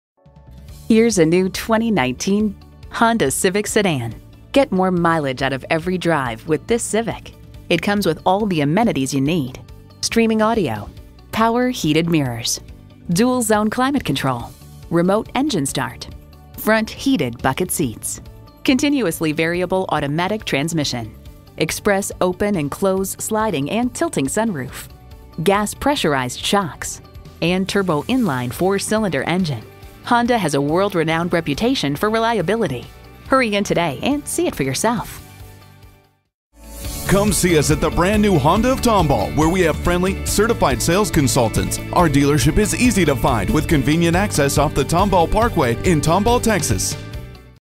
New 2019 Honda Civic Sedan Tomball TX Houston, TX
Year: 2019
Make: Honda
Model: Civic Sedan
Trim: EX
Engine: 4 Cyl. 1.5 L
Transmission: Variable
Color: Silver
Stock #: HTKX008480
VIN: JHMFC1F3XKX008480

Address:
22727 Tomball Pkwy